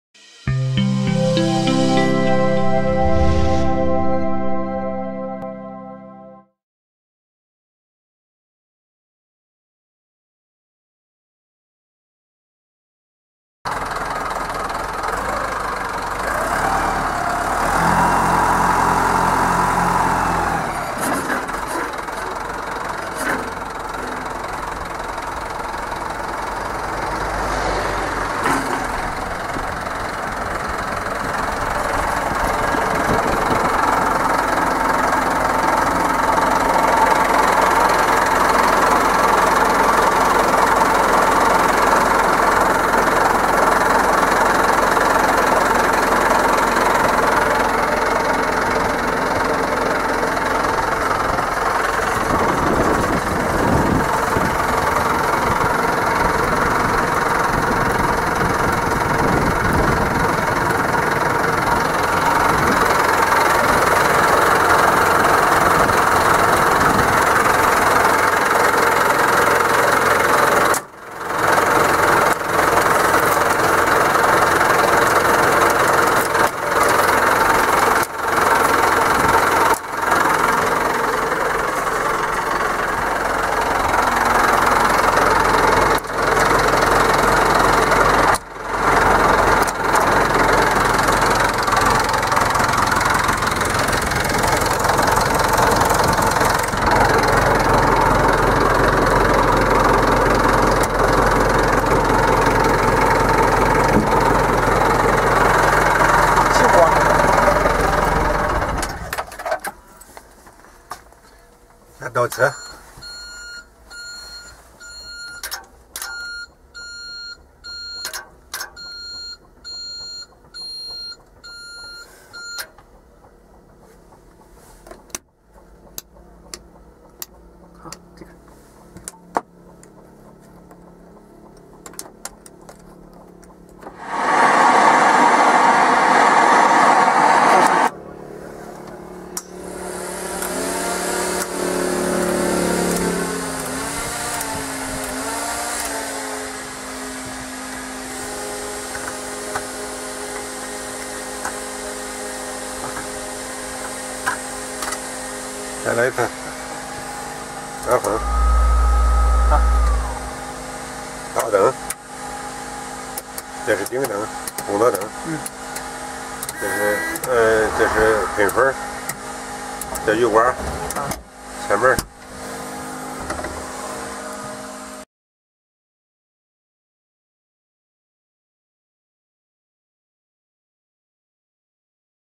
Обзор фронтального погрузчика Everun ER15
Колесный погрузчик Everun ER15 с грузоподъемностью 1.5 тонны отлично подойдет для осуществления погрузочно-разгрузочных работ в сельском хозяйстве, ЖКХ-отрасле, при выполнении общестроительных задач. Машина имеет высокие рабочие характеристики, богатую комплектацию, комплектуется надежным и неприхотливым дизельным двигателем Xinchai.

ООО «Альфаран» - официальный дилер Everun на территории Республики Беларусь.